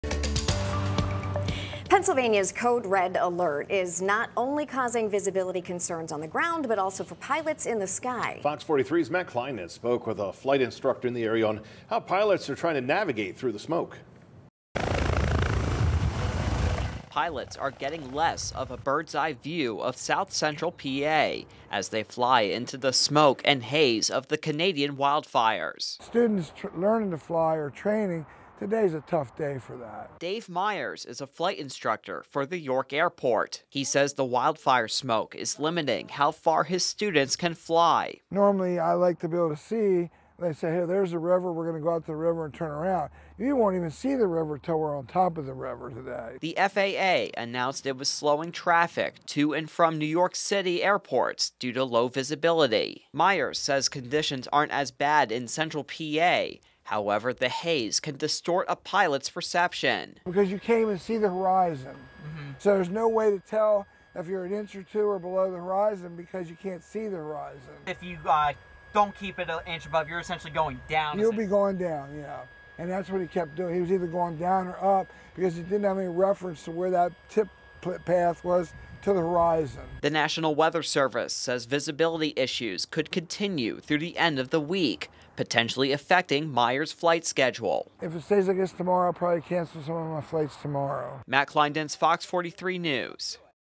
Canadian wildfire smoke impacts visibility for Pa. pilots
The National Weather Service says the smoke and haze of the Canadian wildfires are impacting visibility across the Commonwealth. The visibility issues are being observed on the ground, as well as by pilots in the sky.

“Normally I’d say, ‘We’re going to go out to the river and come back.’ You won’t even see the river until you’re on top of it," said Dave Myers, a flight instructor at the York Airport.

Myers said he's had to limit the distance his students are able to fly, due to the conditions. He adds the weather conditions make it difficult to teach his students.

"For instrument pilots, the corporate guys, [limited visibility] won't make much of a difference," said Myers. "But for a student learning how to fly or for my training, it's really hard."